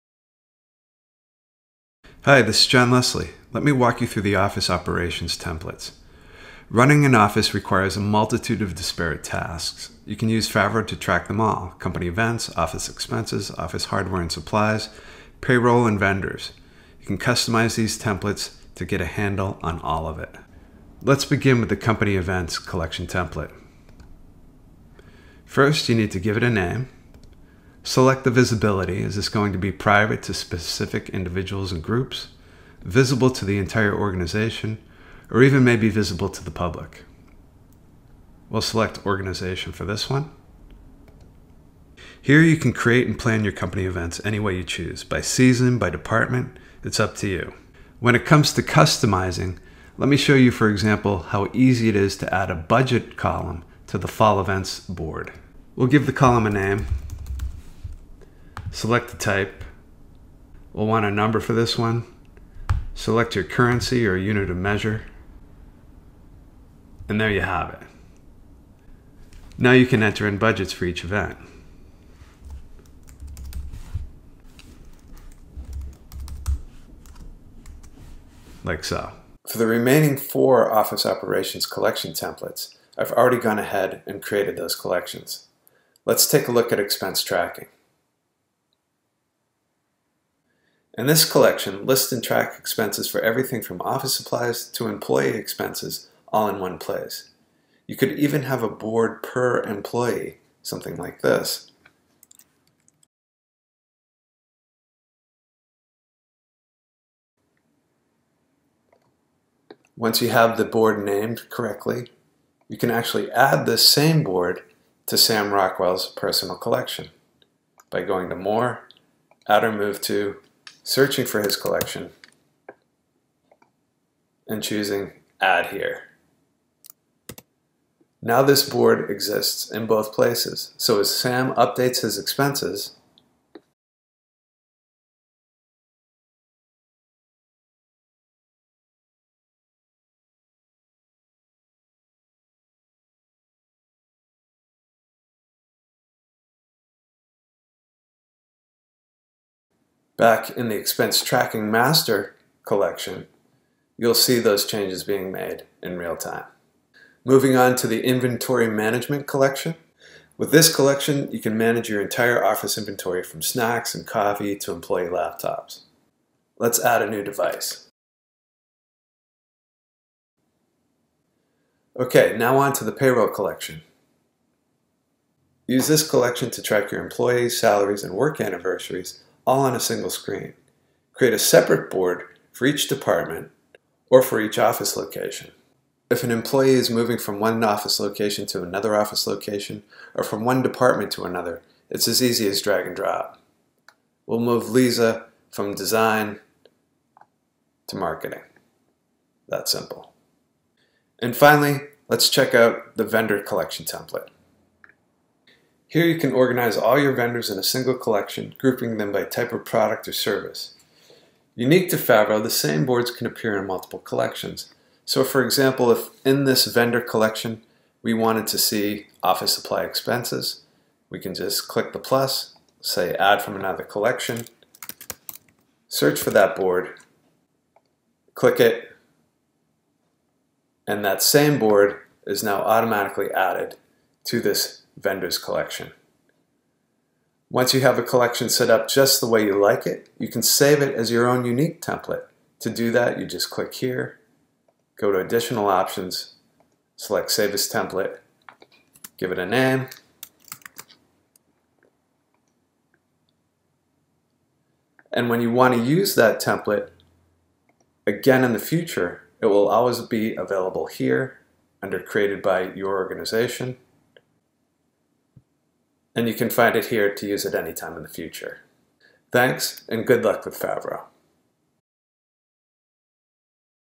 Favro Office Operations Templates
Jon Leslie walks you through the Favro Office Operations Templates.